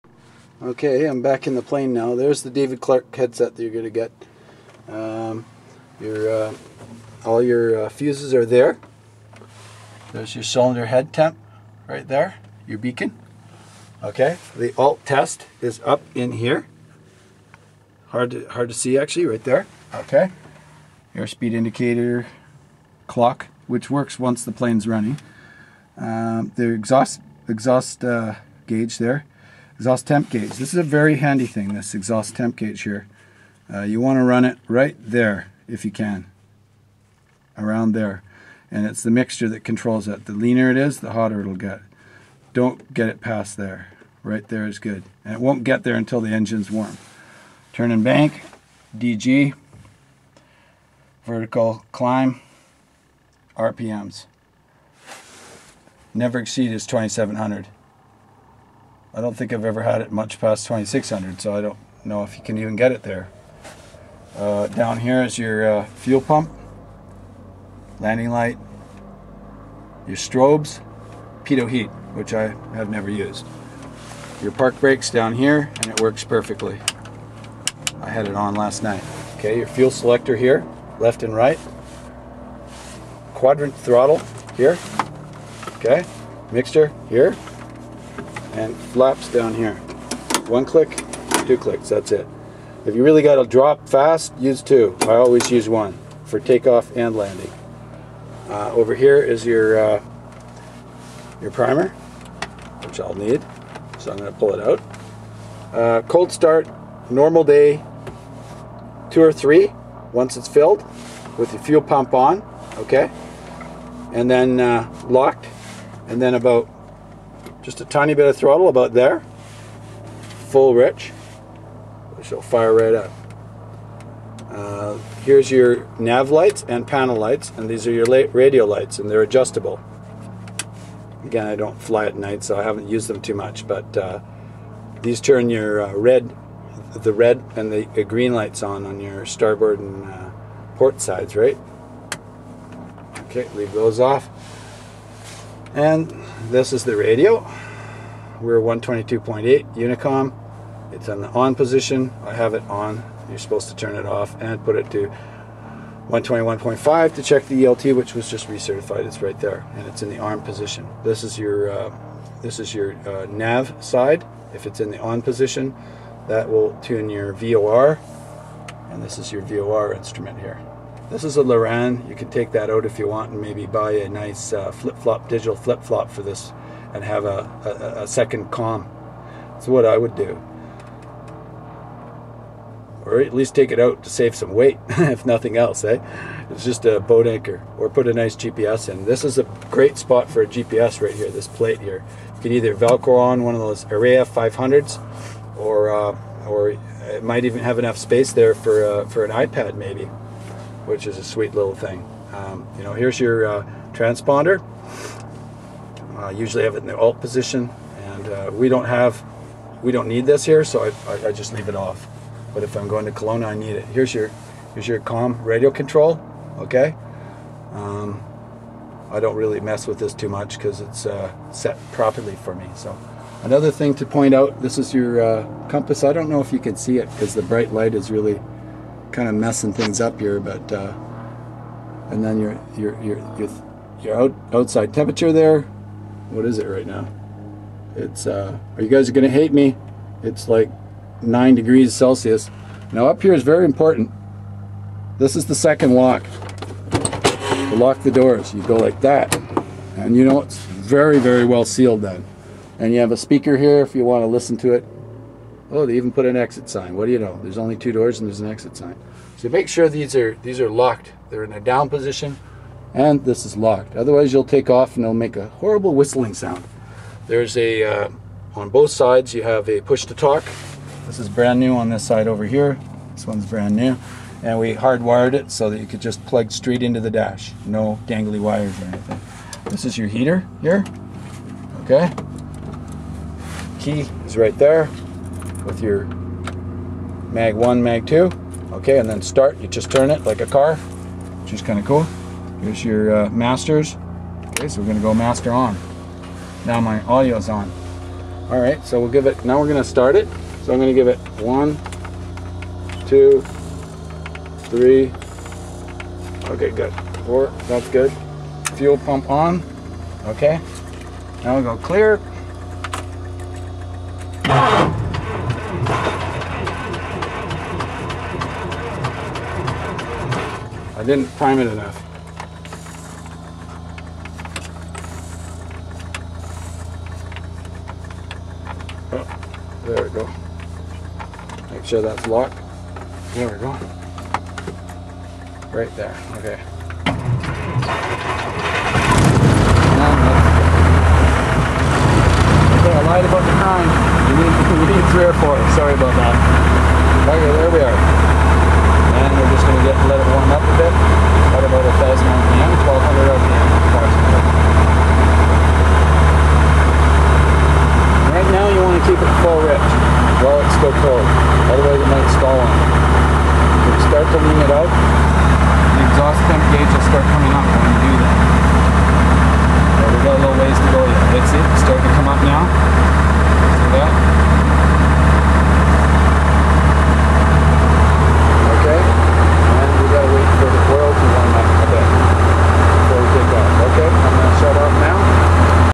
Interior gauges and instruments my piper tomahawk - GHYL
This is a video showing the interior - of my piper tomahawk - and start up procedures.